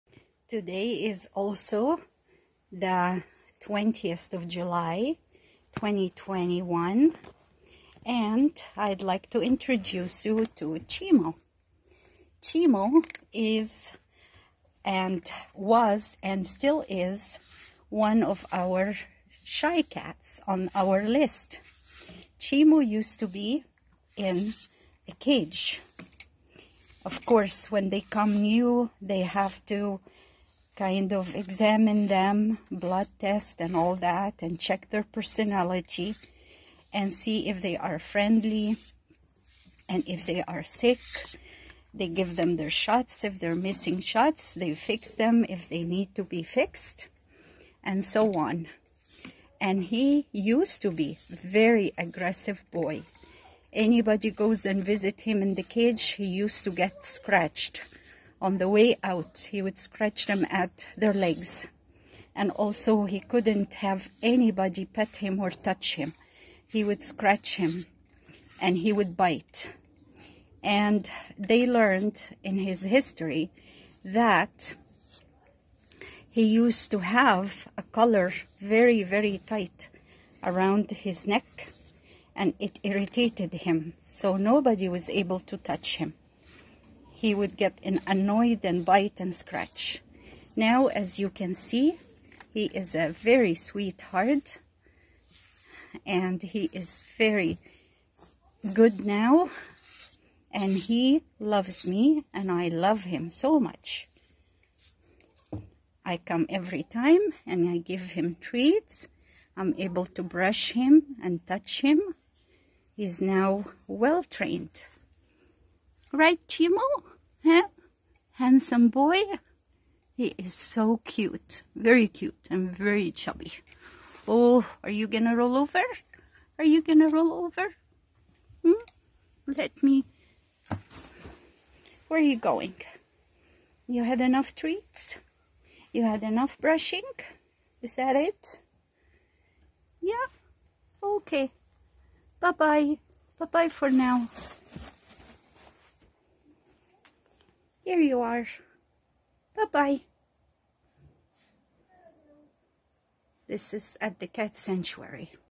Chimo in the back yard at the Cat Sanctuary
July 20th 2021  Chimo in the back yard. Chimo was adopted in January 2022. I Shared his story on the video while brushing him.